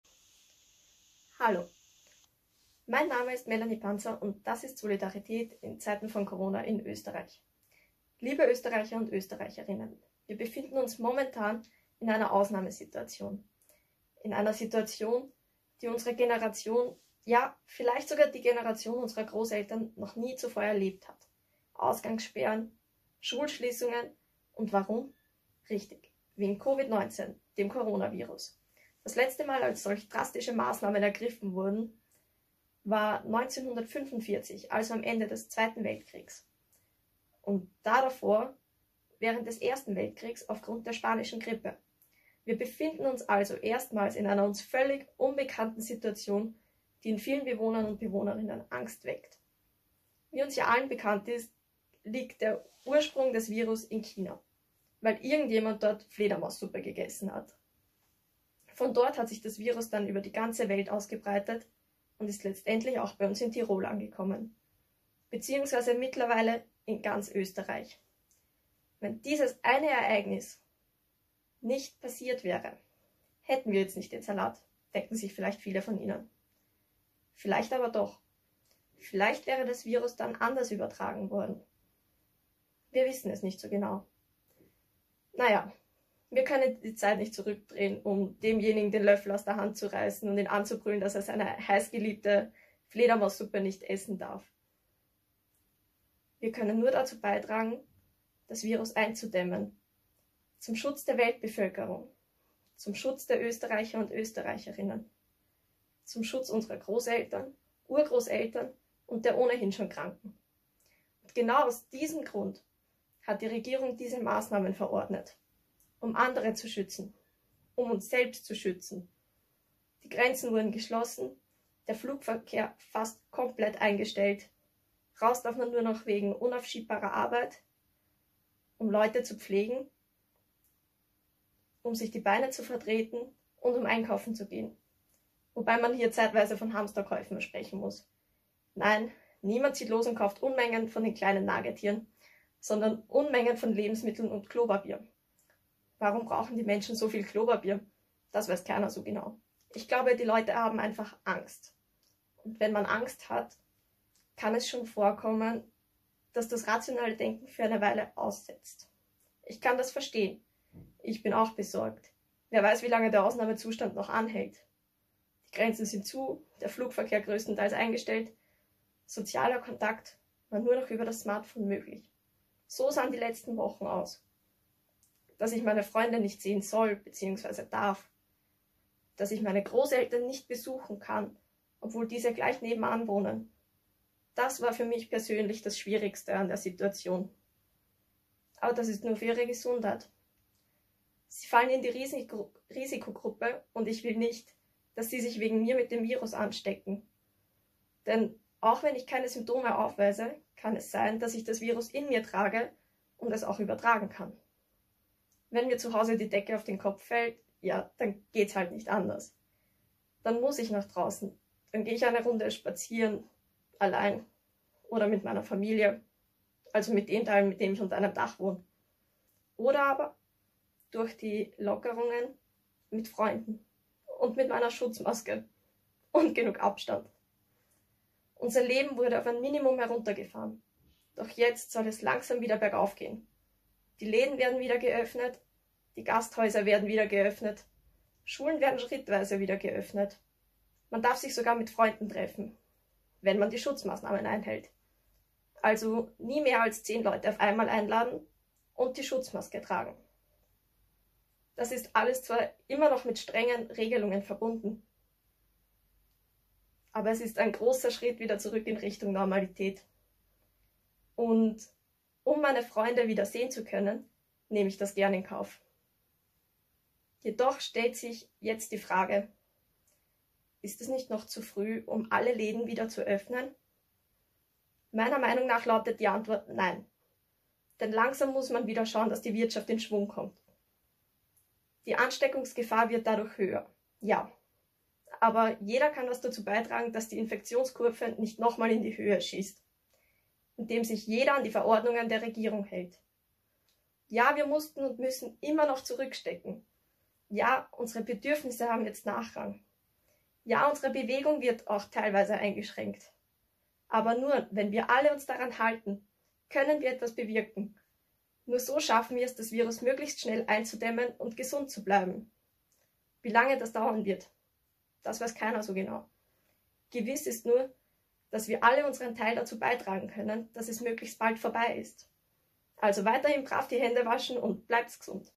HTL SchülerInnen zum Thema Covid-19
Teil 3 unserer kleinen Serie: Im Rahmen des Deutsch-Unterrichts verfassen die SchülerInnen der IV Jahrgänge Meinungsreden zum Thema Covid-19.